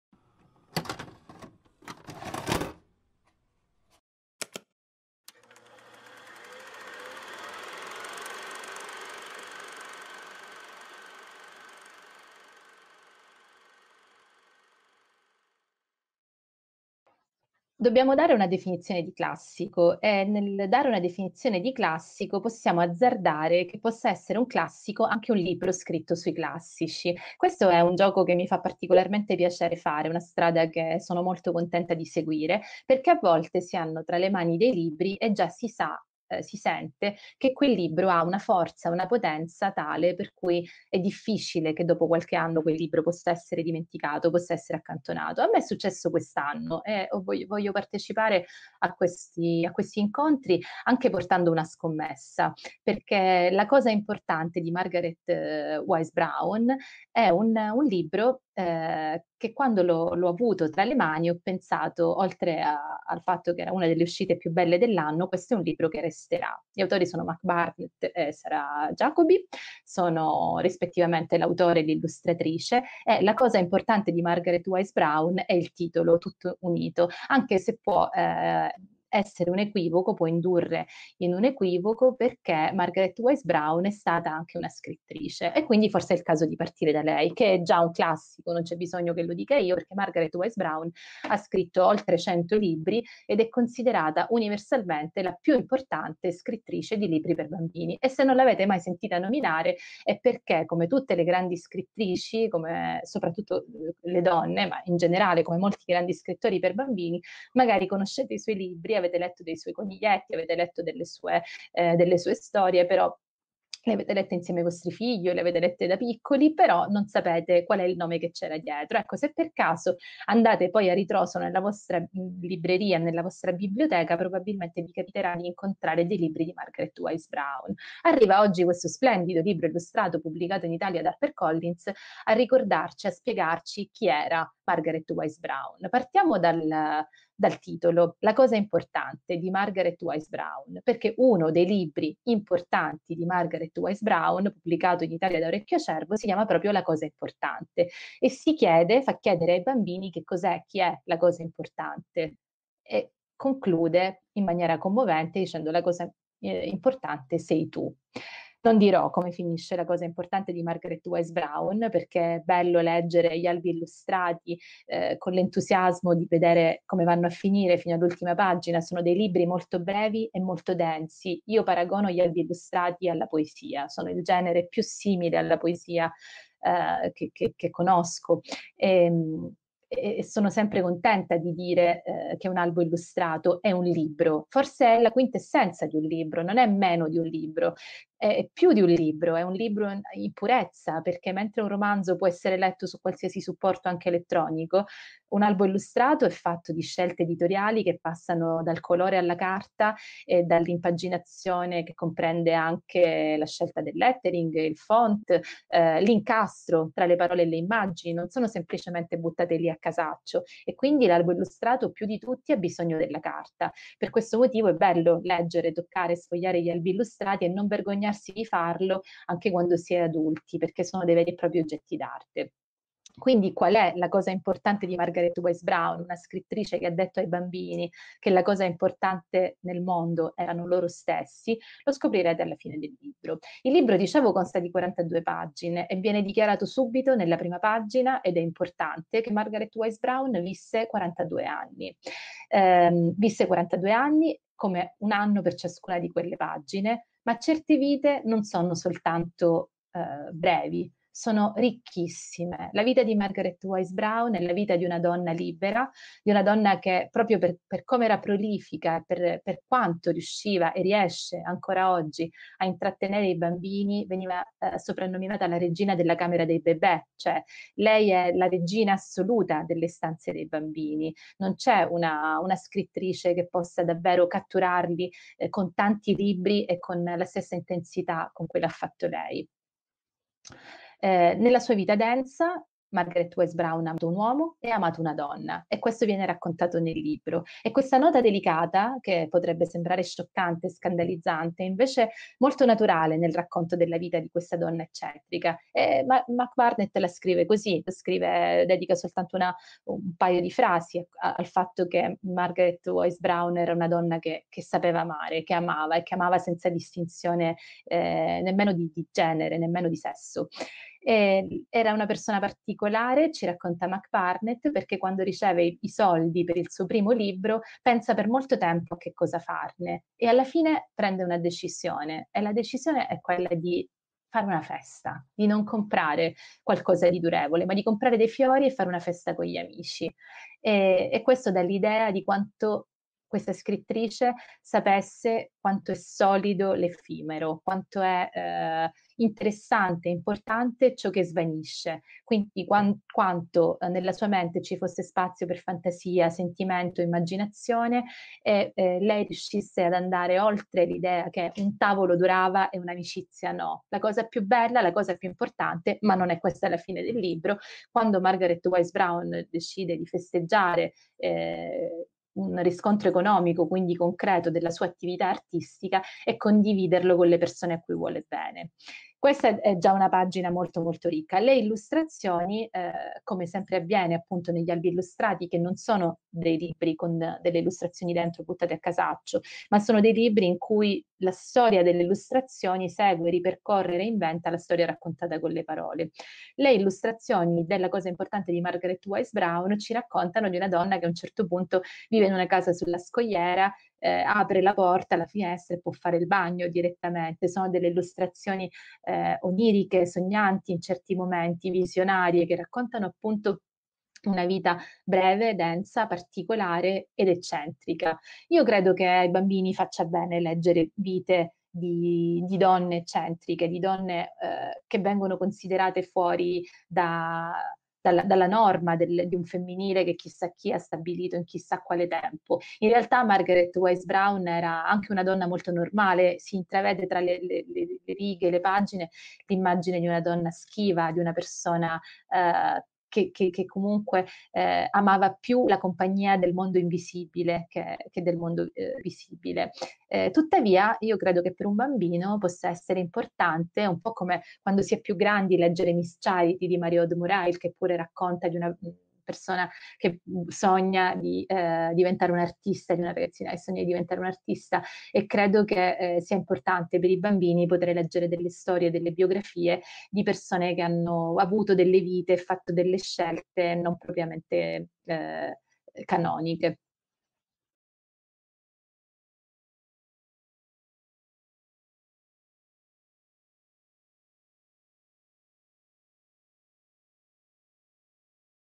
Piccoli classici #2 / Nadia Terranova
Quattro libri per bambini (che piacciono anche agli adulti) scelti da Nadia Terranova, perché leggere significa conoscere nuovi amici, giocare, volare, sognare, fare viaggi lunghissimi.

#2: "La cosa importante" di Margaret Wise Brown